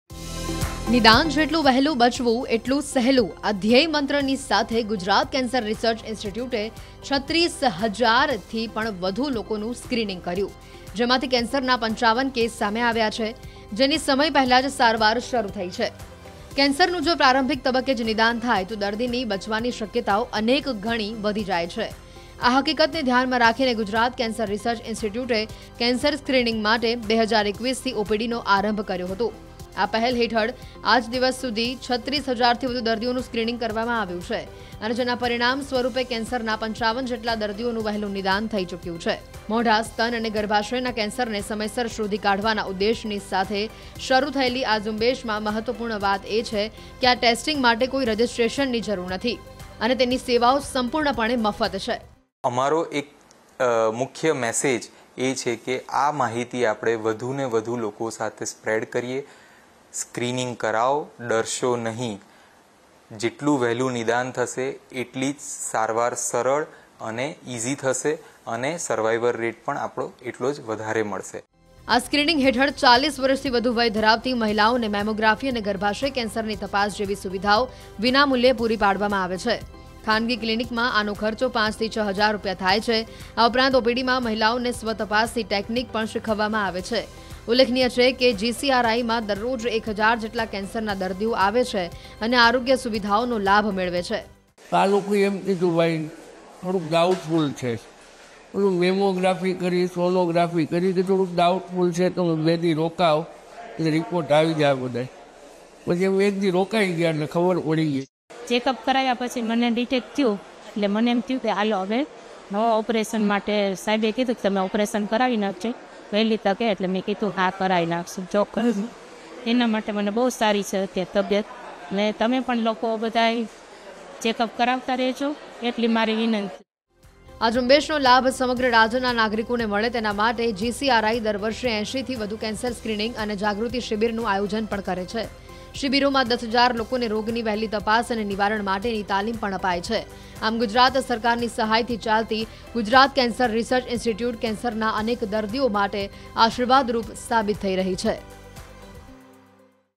Ahmedabad News | કેન્સર છે કે નહીં એની સરકાર મફતમાં કરી આપે છે તપાસ | Hospital| Gujarat|News 18|N18V
Gujarat Cancer Research Institute screened more than 36,000 people with the motto "The earlier the diagnosis, the easier the survival". Out of which 55 cases of cancer have been detected... which were treated prematurely.

નિદાન જેટલું વહેલું બચવું એટલું સહેલું આ ધ્યેય મંત્ર સાથે ગુજરાત કેન્સર રિસર્ચ ઈન્સ્ટીટ્યૂટે 36 હજારથી વધુ લોકોનું સ્ક્રિનિંગ કર્યું.. જેમાંથી કેન્સરના 55 કેસ સામે આવ્યા છે... જેની સમય પહેલાં જ સારવાર શરૂ થઈ.n18oc_surat